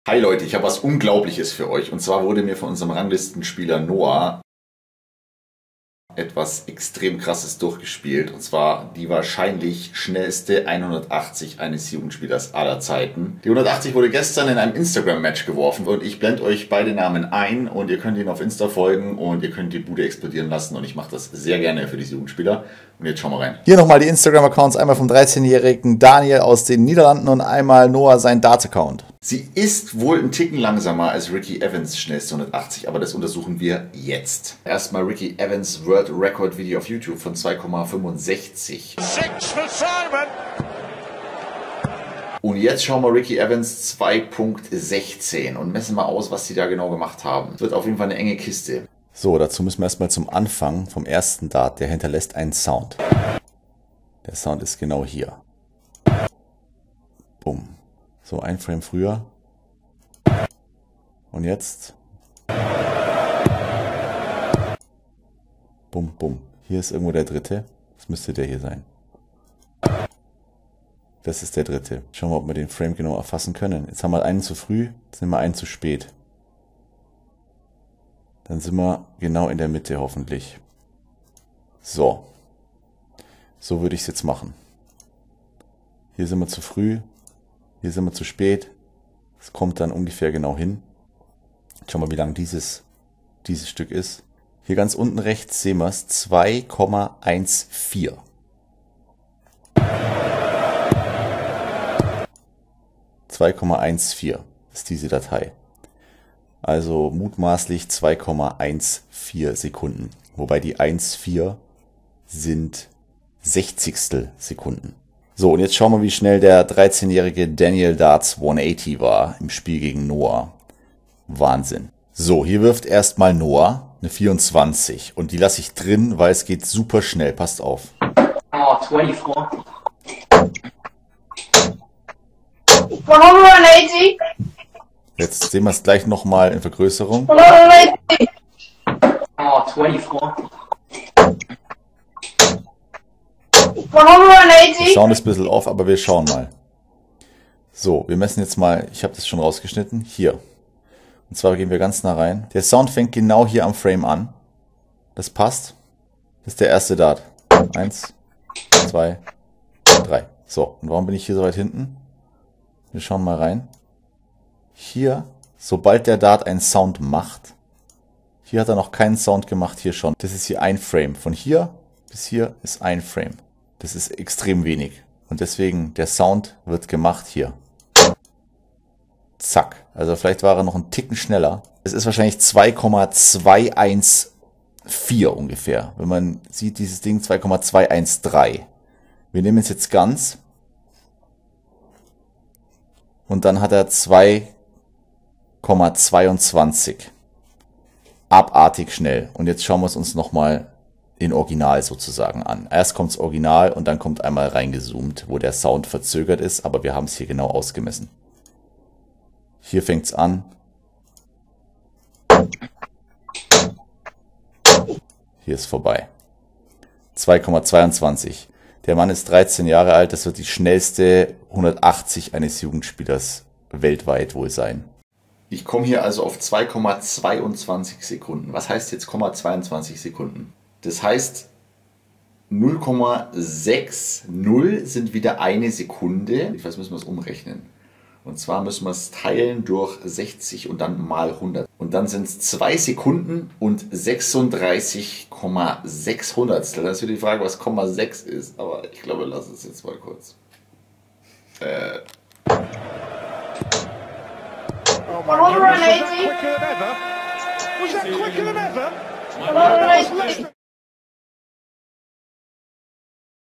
Jugend WELTREKORD: Schnellste 180? Mit Analyse. Nicht blinzeln!
Ich glaube auf Insta wurde gerade eine 180 in Weltrekordzeit geworfen und zwar von dem 13-jährigen Niederländer @Danieldarts180. Wir schauen uns das Ganze an.

Vielen Dank an Insta @durchnernoah7 und seinen 2t Account über Darts @dart_verrueckt_180 der diese 180 im Spiel gegen Daniel kassierte.